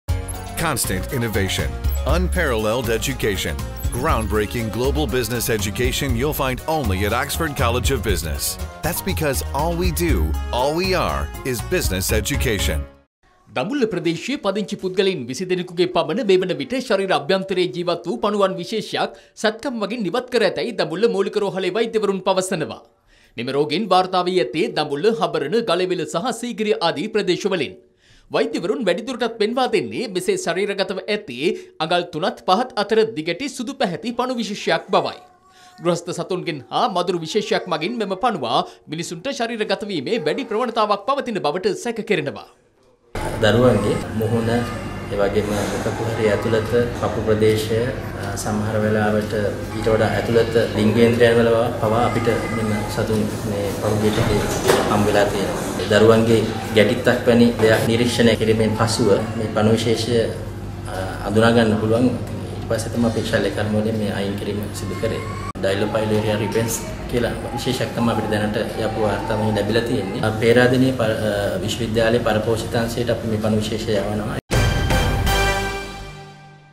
Species of worm removed from over 20 patients in Dambulla
දඹුල්ල ප්‍රදේශයේ පුද්ගල ශරීරගත වූ පණු විශේෂයක් සොයා ගැනේ...